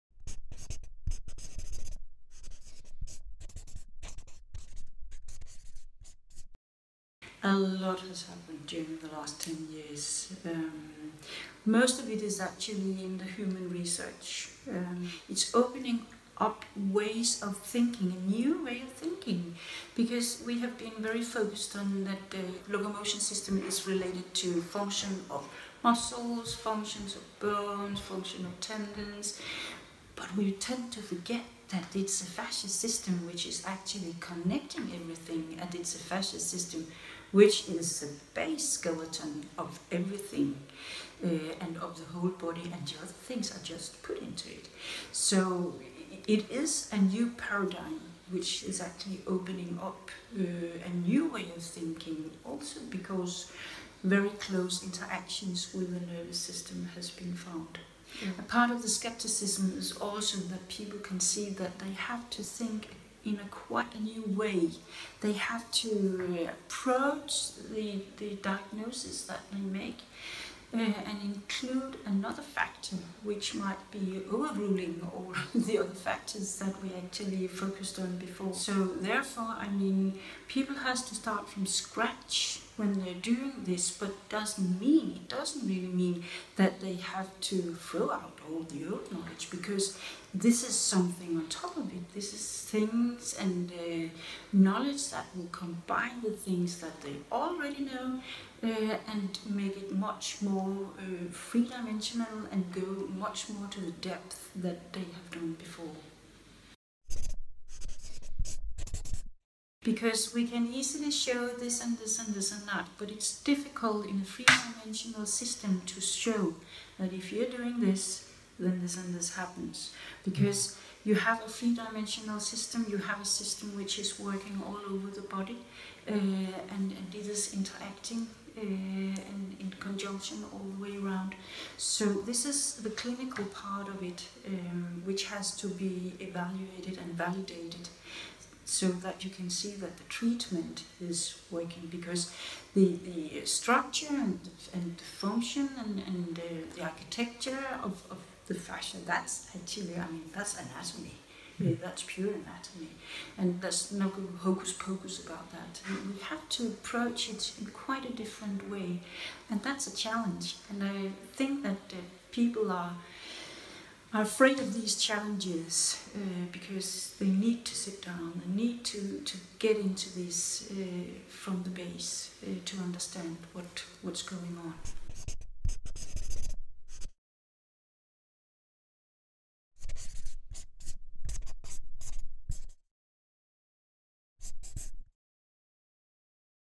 Fascia: Vad har hänt inom anatomisk forskning de senaste 10 åren?
2015 blev veterinär Vibeke S Elbrønd först i världen med en publikation om Fascia på hästar. För att kunna göra det blev hon tvungen att sätta sig in i all forskning som fanns om Fascia. Så vi frågade denna Fascia-expert: Vad har hänt de senaste 10 åren? Svaret gjorde vi en film av